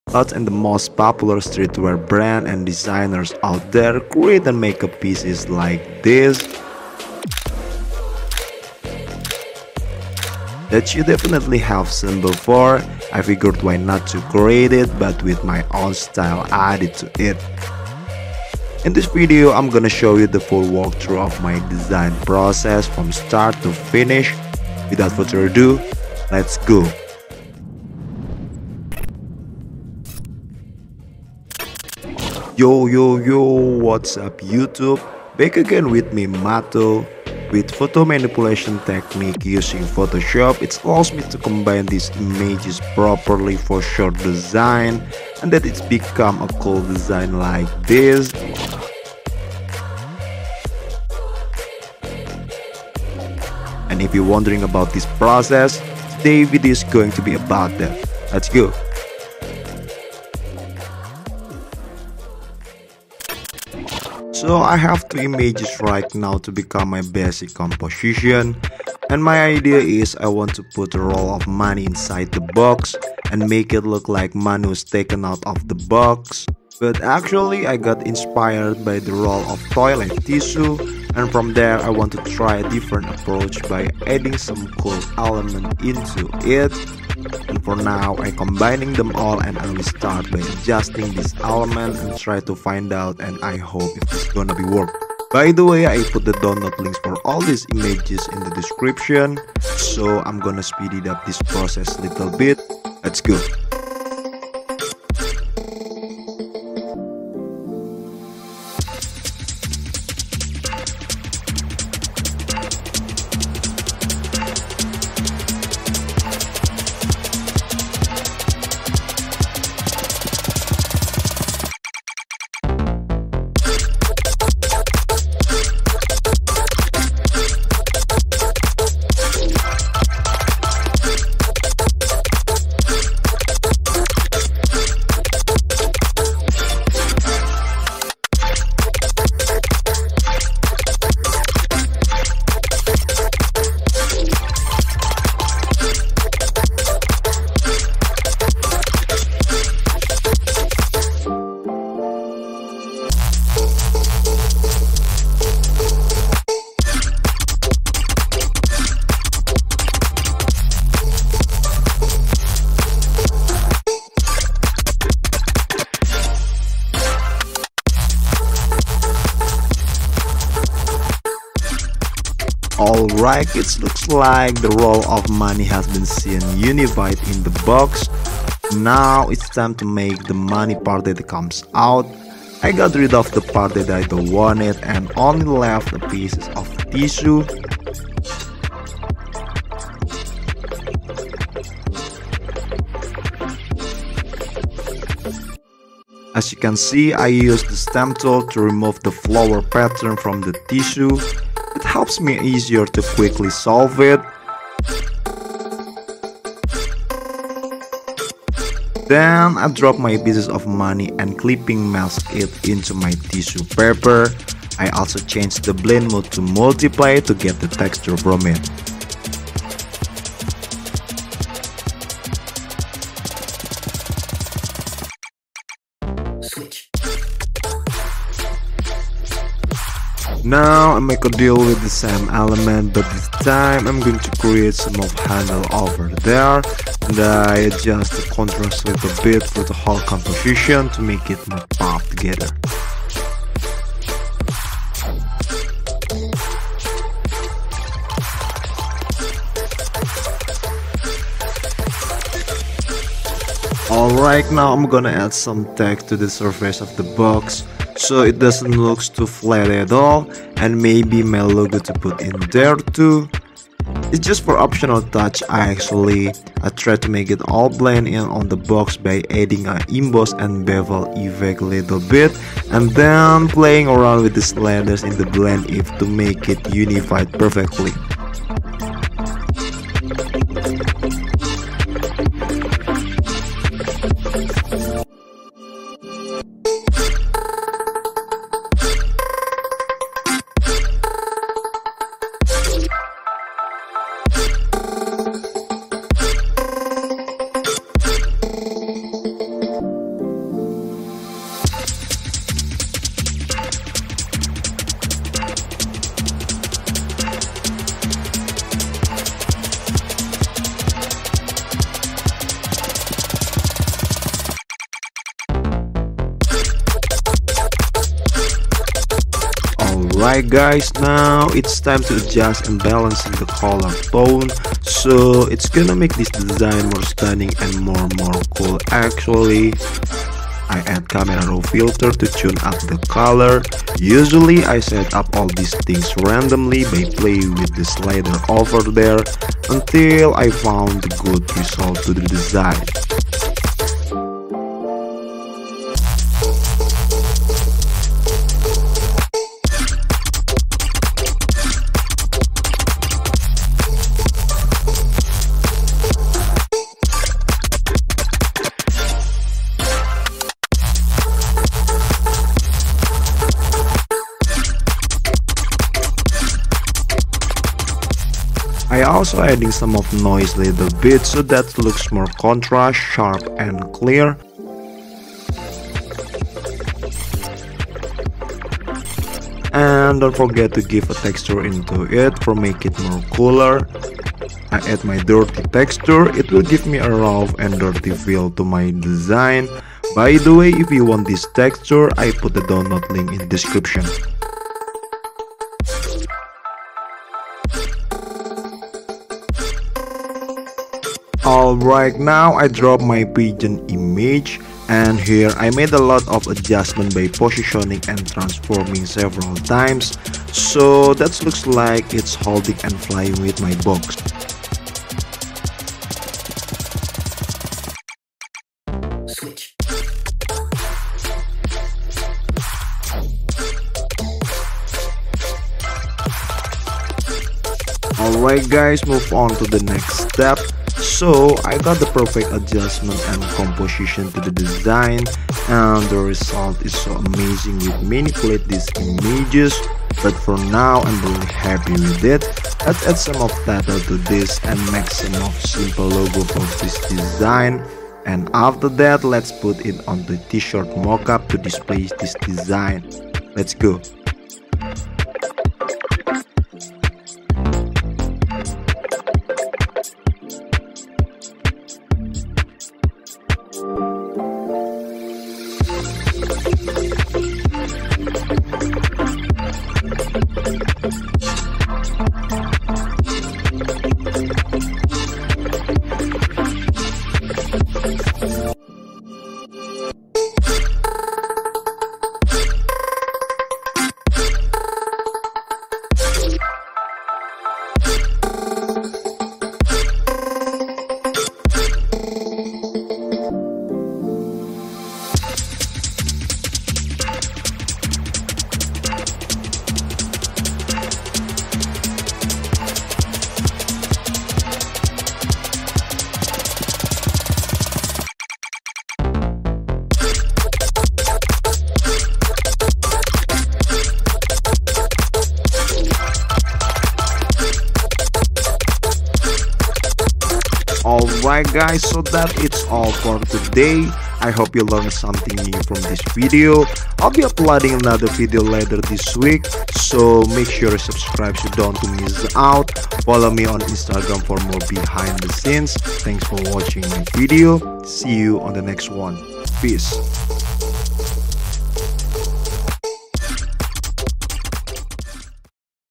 Manipulating Images For Streetwear T-shirt Design | Photoshop Process Design
In this video, I will designing an original Merch Design into realistic way using Adobe Photoshop! Combine several elements into one, resulting a unique designs for a tshirt.

its makes a difference. i really appreciate for your support guys.